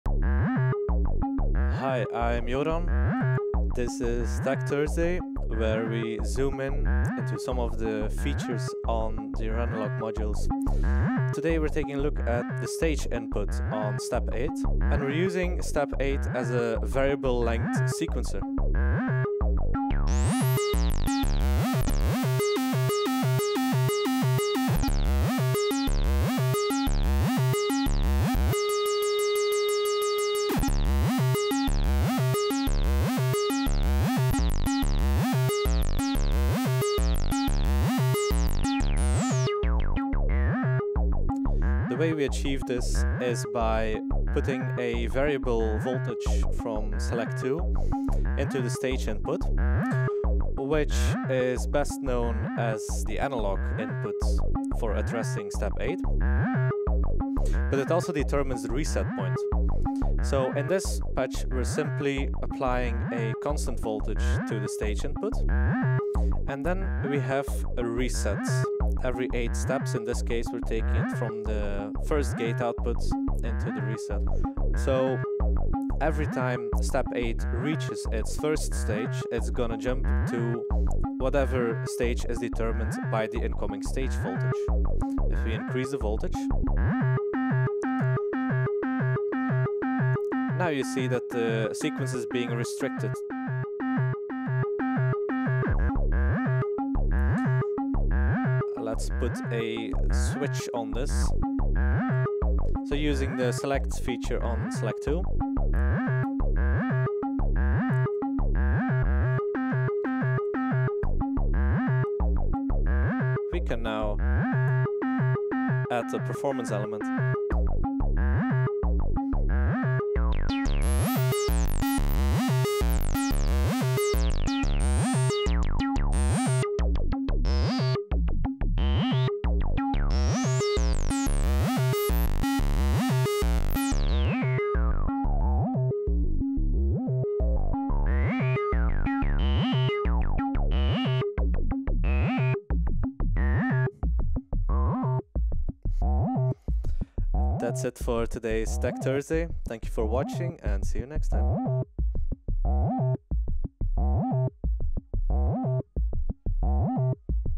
Tech Thursday | Step 8 as a Variable Length Sequencer
Besides bringing back Patch Tuesday, we're also giving you Tech Thursdays! In these concise videos, we bring up some overlooked and advanced features of our modules.

This week: Step 8 as a variable length sequencer, powered by the stage CV input. Did you know that in addition to directly addressing the register by CV, this input also allows you to change the reset point? By patching Step 8 to reset itself, we can thus dynamically vary the length of the sequence under voltage control.